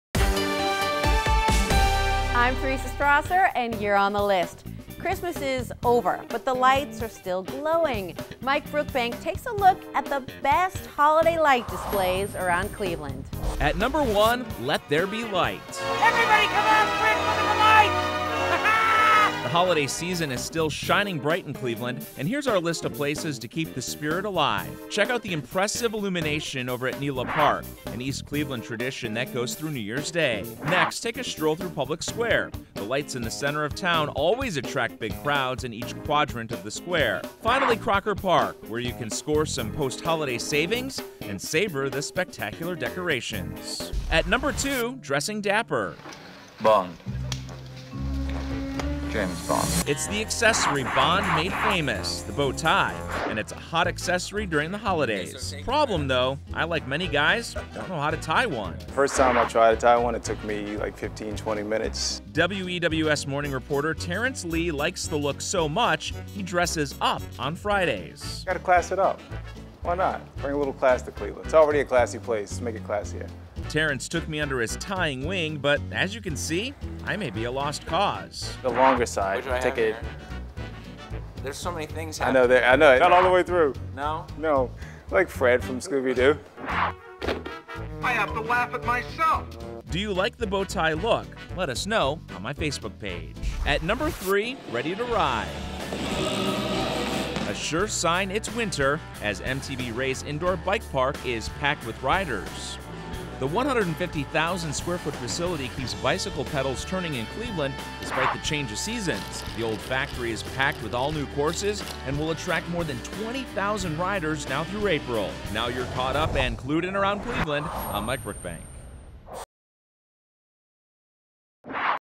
AROUND CLEVELAND: Best places for Christmas lights, dressing dapper and bike year round
Christmas is over, but the lights are still glowing. Here's a list of the best holiday light displays in Cleveland.